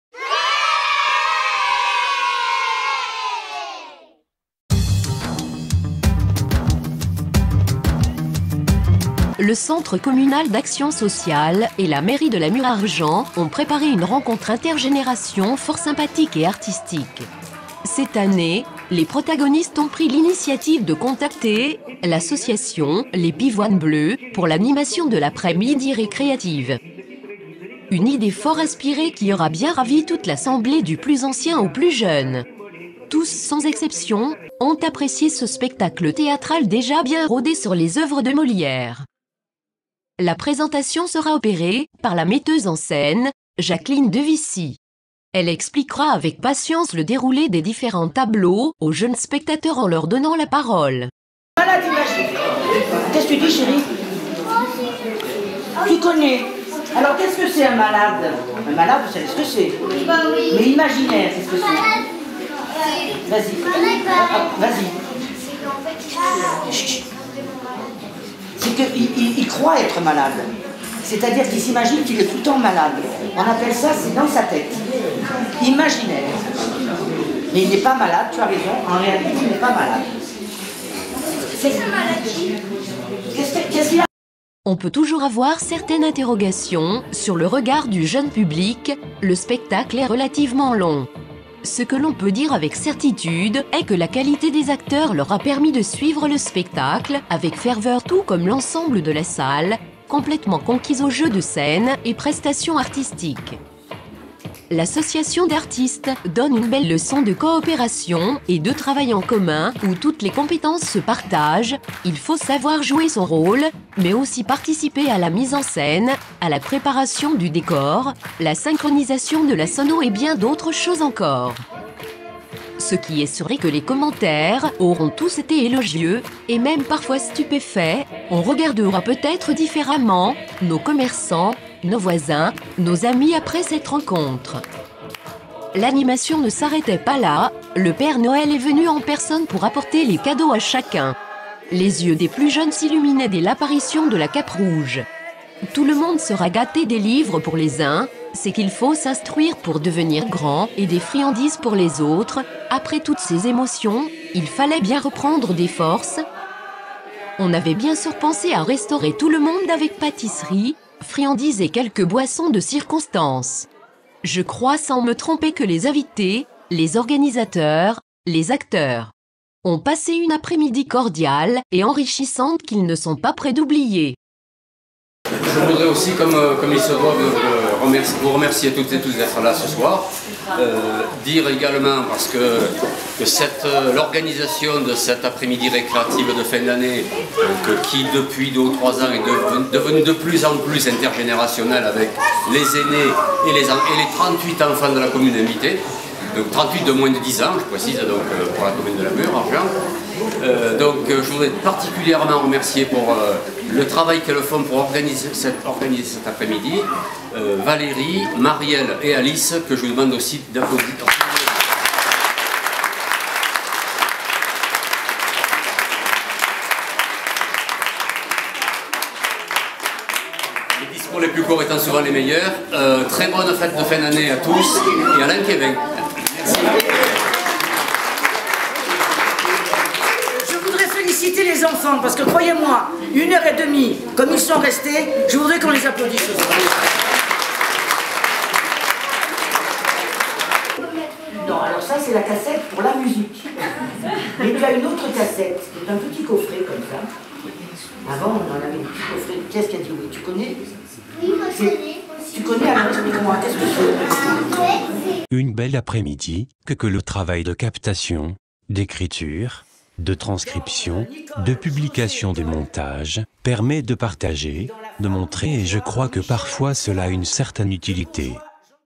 spectacle gouter de Noel La Mure Argens
Vidéo de Présentation de l'animation de Noel pour le gouter intergérationnel de la Mure Argens